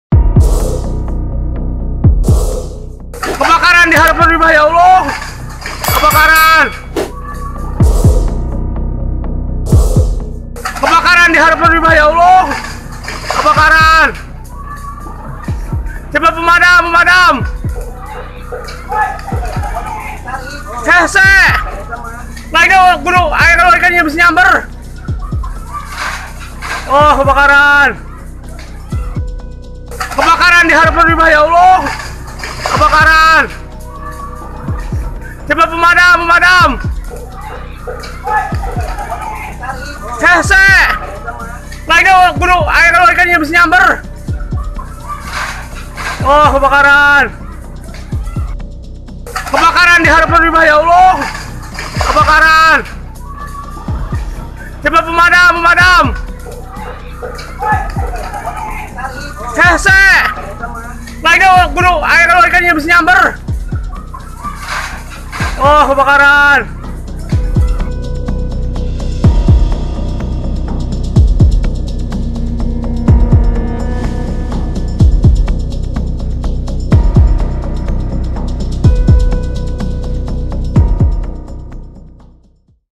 Waduh! Sebuah Odong-odong dan 1 Unit Rumah di Parung Panjang Hangus Dilahap Si Jago Merah
Sebuah Odong-odong & 1 unit rumah di Kp Pabuaran, Desa Gorowong, Kecamatan Parung Panjang.

Odong-odong dan rumah tersebut terbakar pada hari Kamis (6/10/2022) siang.

Diketahui, kebakaran disebabkan oleh percikan api las yang menyambar ke bensin.

Sebanyak 2 unit Pemadam kebakaran dikerahkan Damkar Parung Panjang & berhasil padamkan api tersebut.

Untungnya, tidak ada korban terluka atau jiwa dalam peristira tersebut.

-YPN-